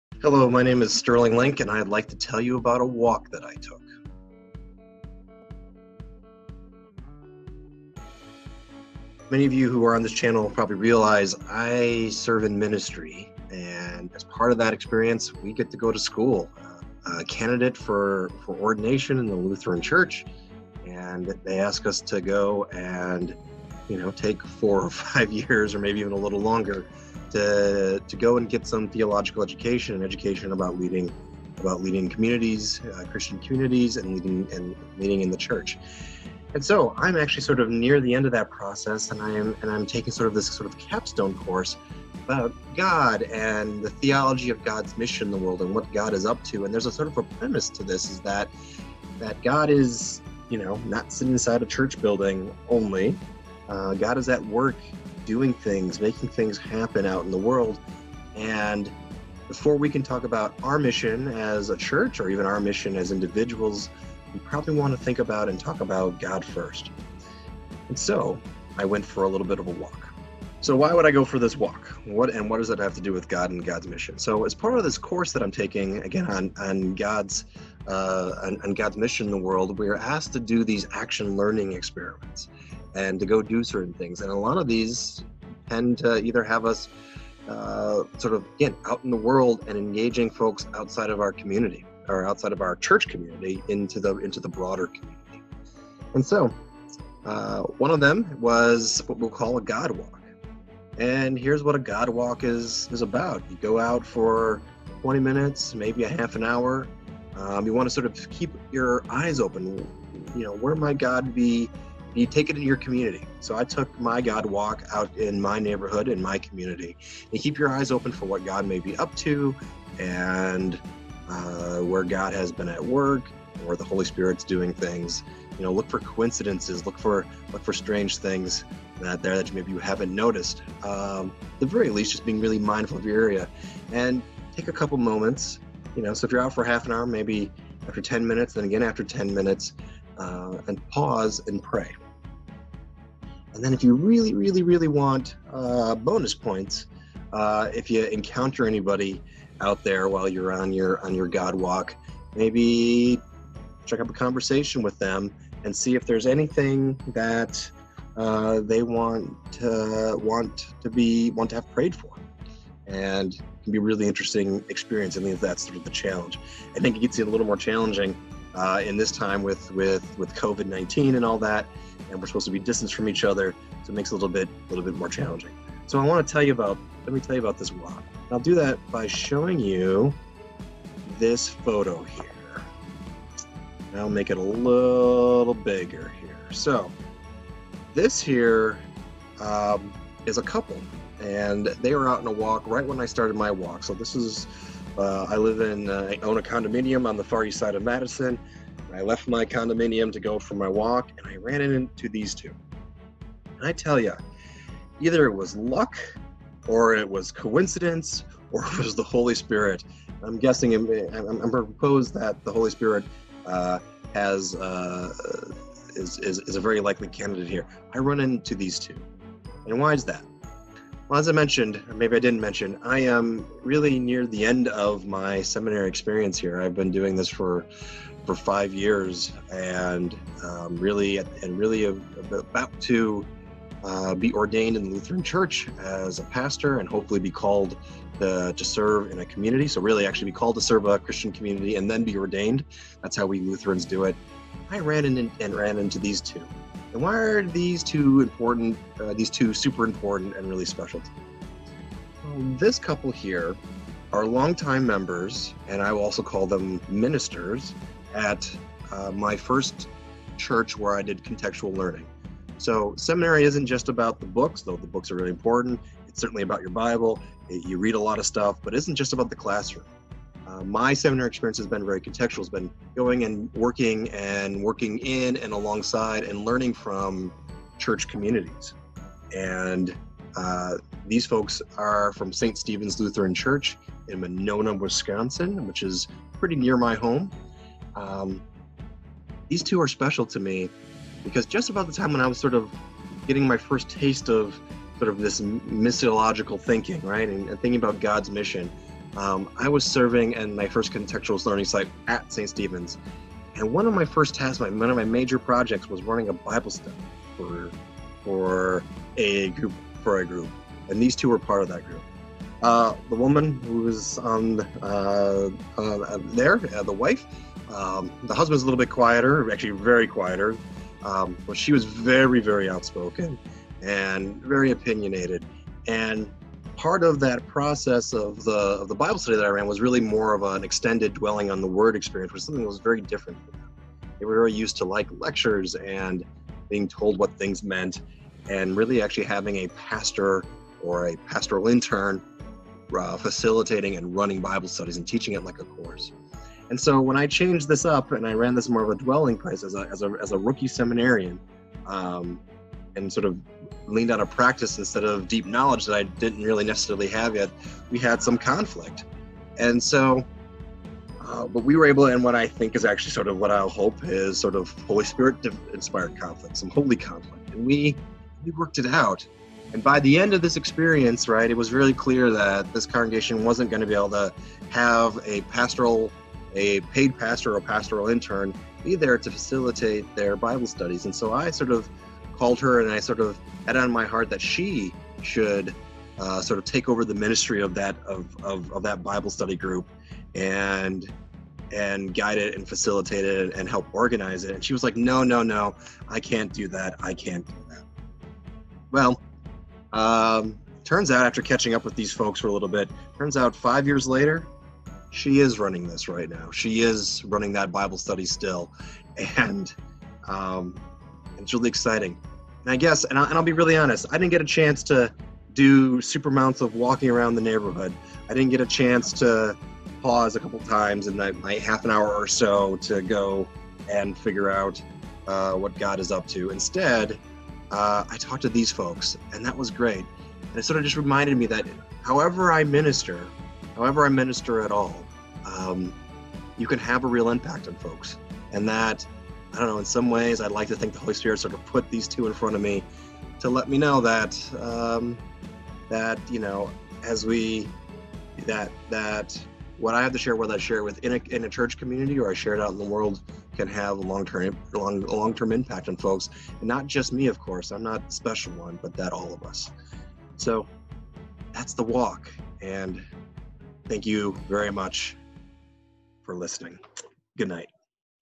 Action Learning Experiment #1
Give this one a try. Here are the instructions that my professor shared with me to do this discernment activity.

Go on a God Walk:  Go on a 20-25 minute walk around the neighborhood near you. Pay attention to anything you think might indicate God’s presence or activity. Take a picture (if appropriate) of any of these “God sightings.” Pause twice on your walk to pray a prayer out loud for the neighbors/neighborhood.

Extra Credit: Try and strike up a conversation with someone on the street and ask them their name and if there is anything you can pray for them.